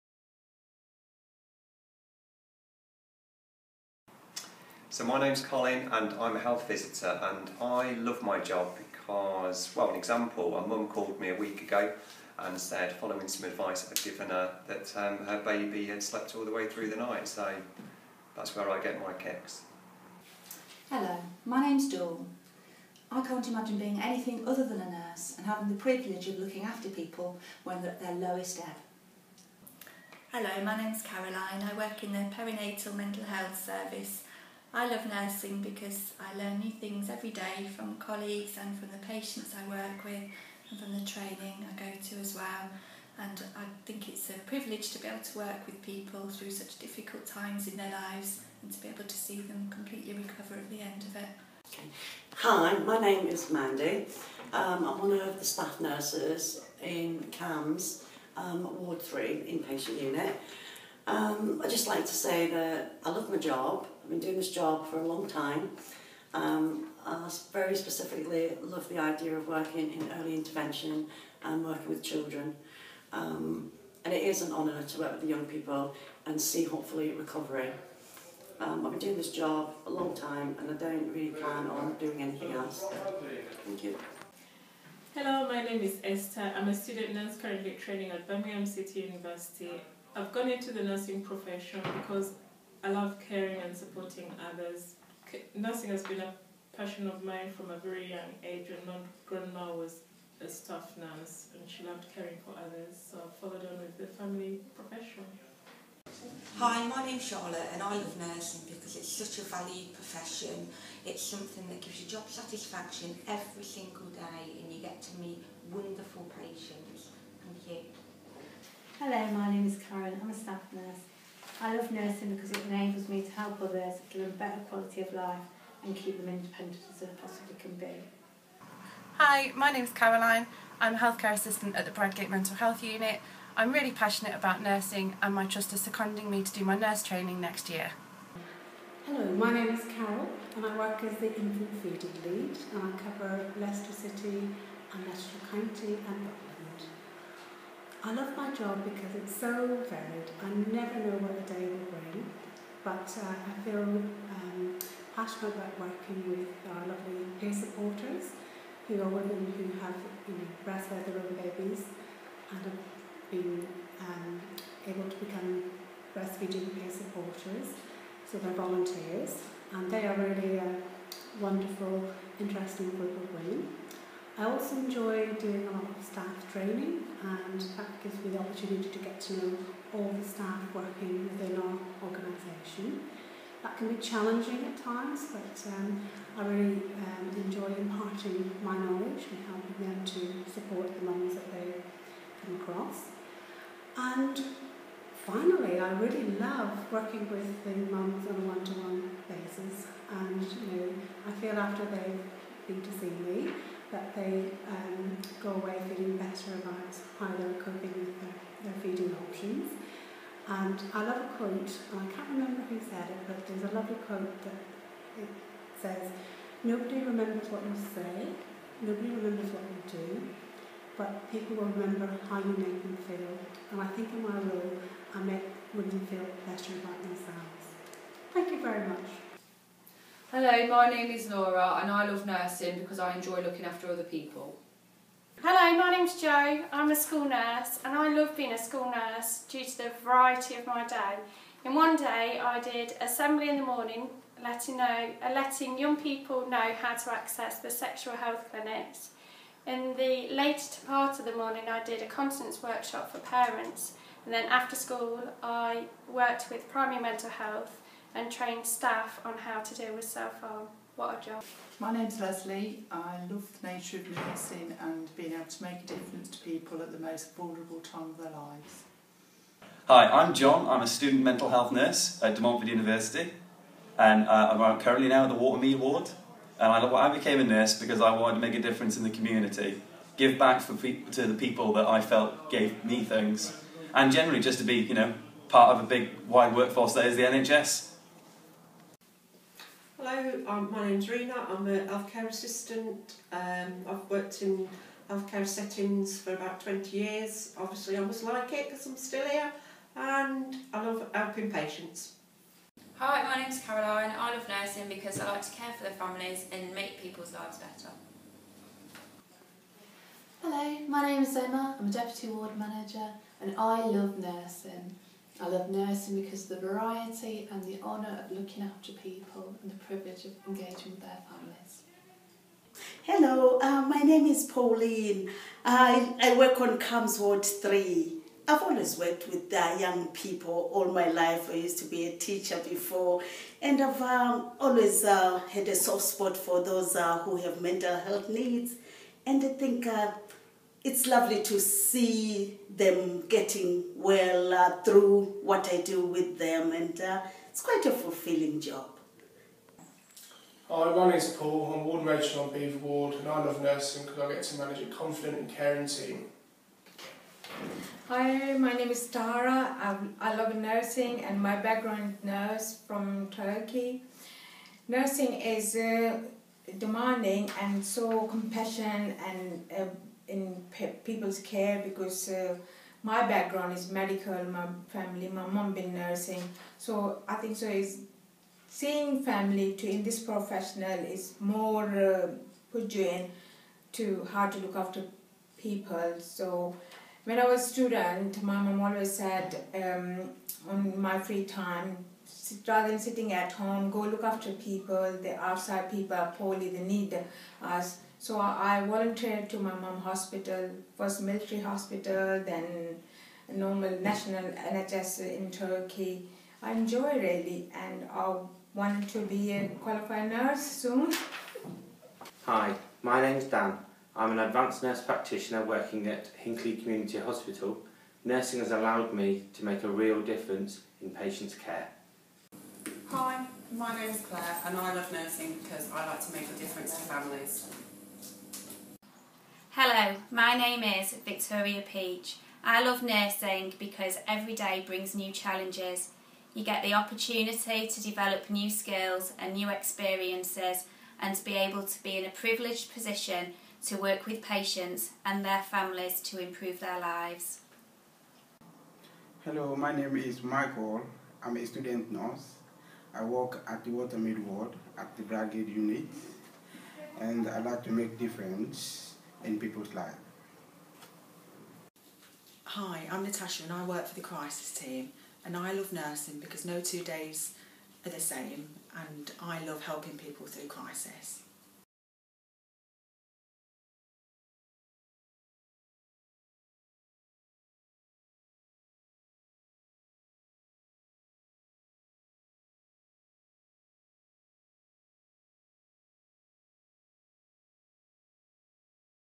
Compilation master
We asked 24 of our nurses what they loved about their jobs, and this is what they said. This was made to mark international Nurses' Day 2016.